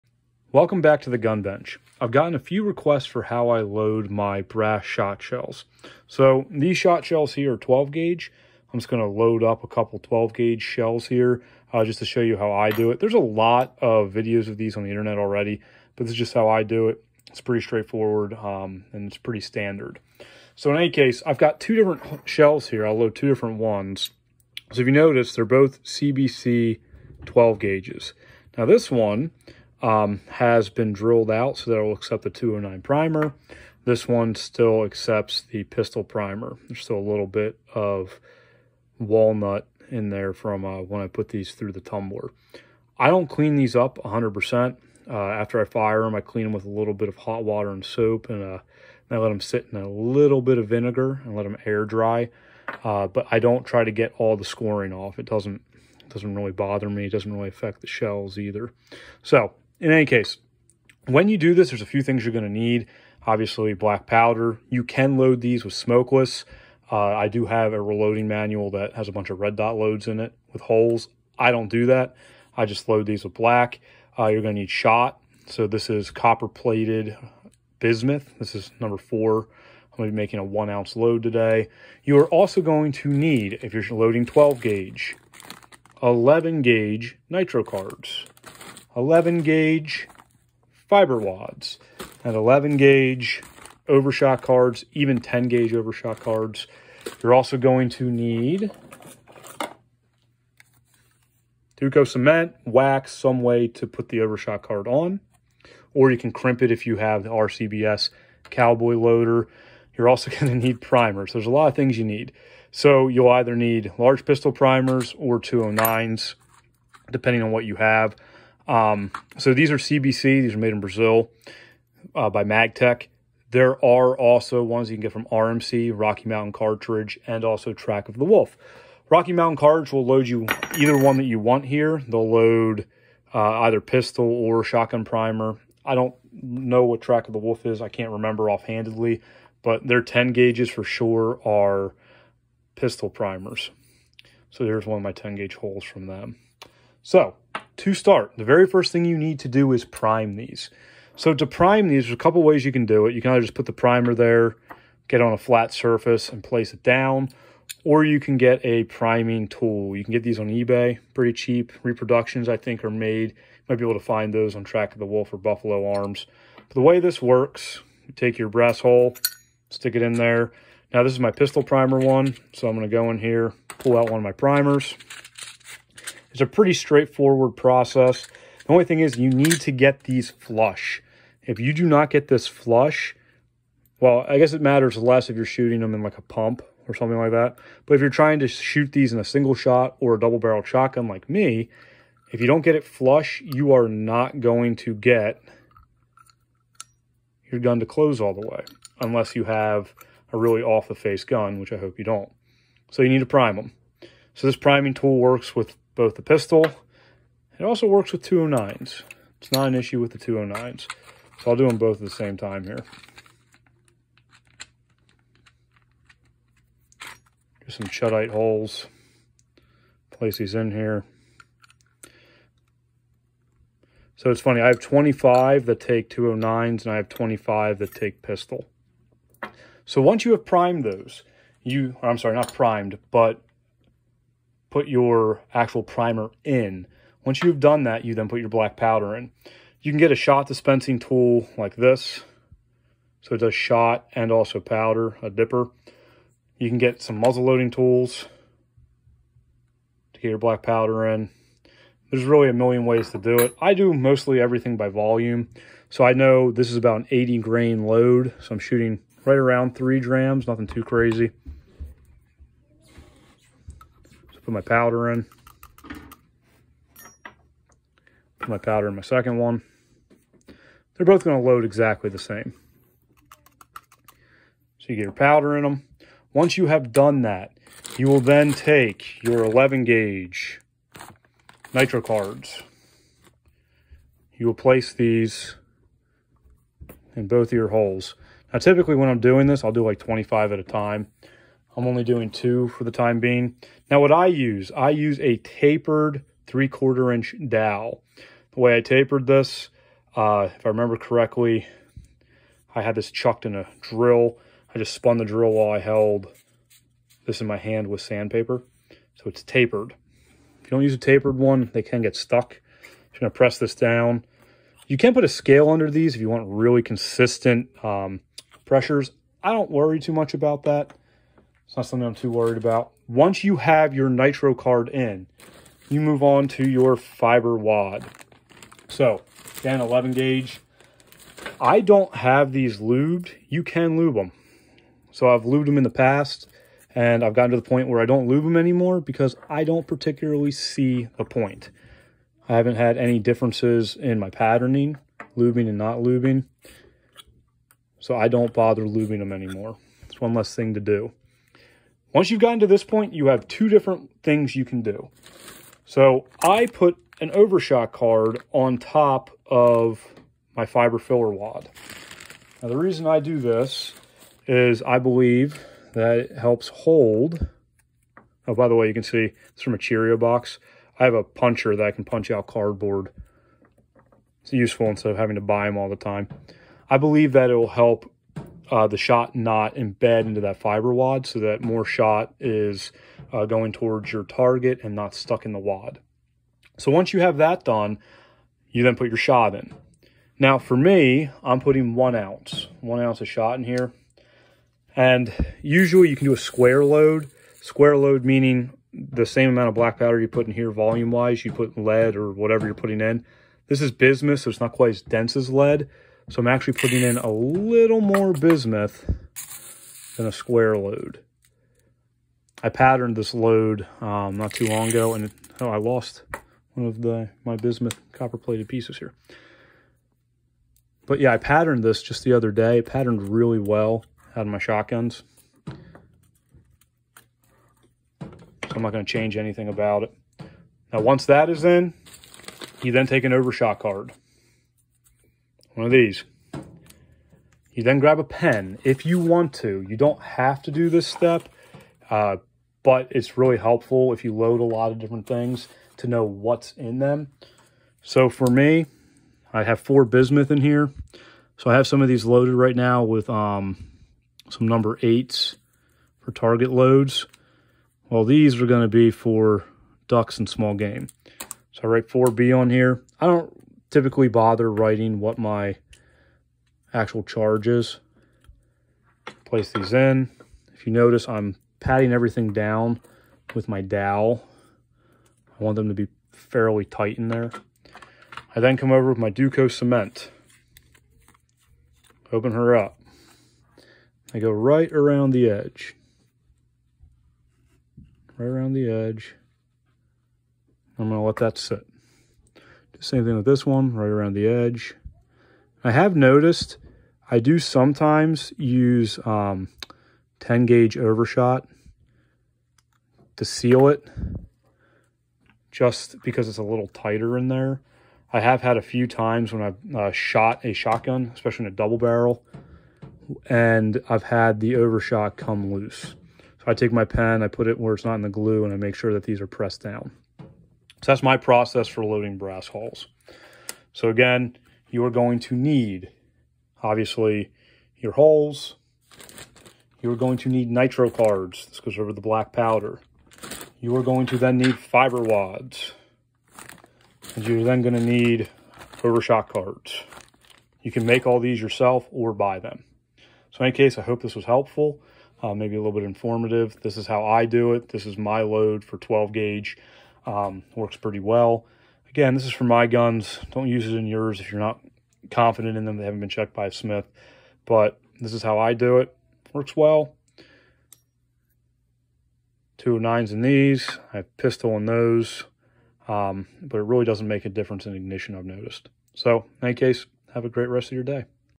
Reloading 12 Gauge Brass Hulls with Black Powder
I’m using Magtech hulls here, but Rocky Mountain Cartridge, and Track of the Wolf also currently make these in multiple gauges. You can also find antique ones that also will work.

If you’re looking for more load data check out Ballistic Products. They have a manual just for brass hulls. Track of the Wolf also has a 10 gauge pamphlet (as well as many of the tools you might need). As always, have your guns checked out by a competent smith before use. Enjoy!